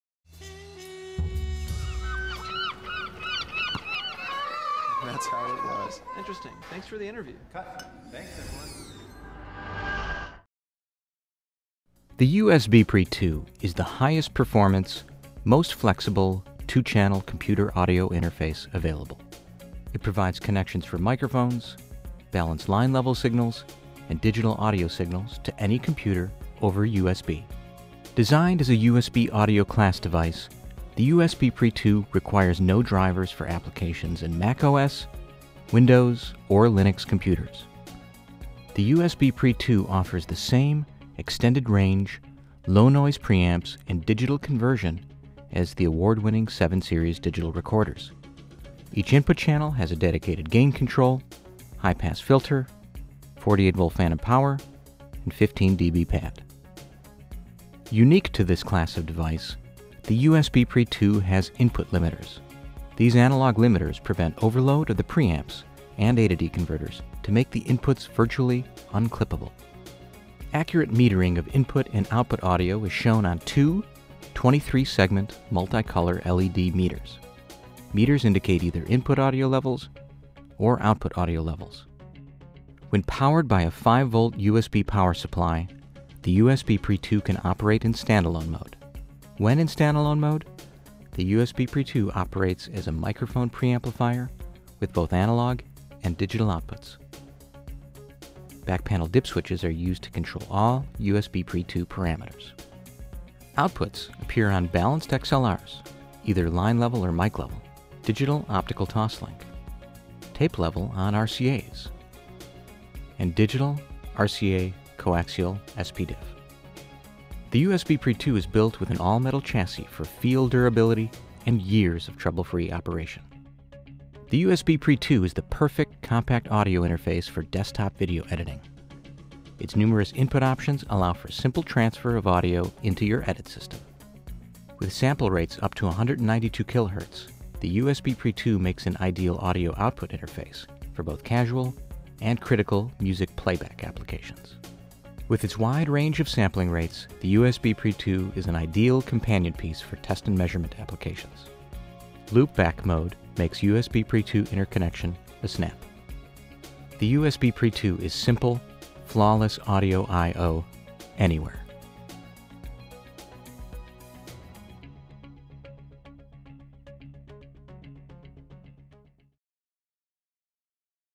USBPre 2 - Product Overview Video
A three minute overview of Sound Devices USBPre 2 Computer Audio Interface